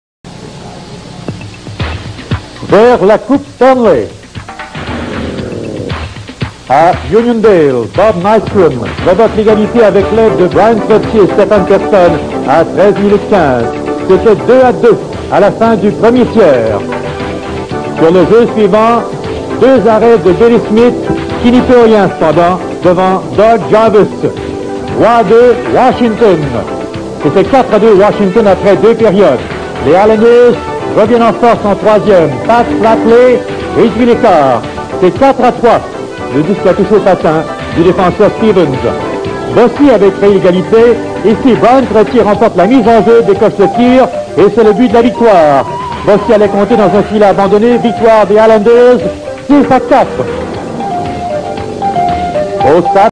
Game 4 1985 Patrick Division Semifinal Capitals at Islanders SRC highlights (FRE)
French language highlights of isles-Caps from the SRC broadcast of MTL-BOS G5.

1985 NHL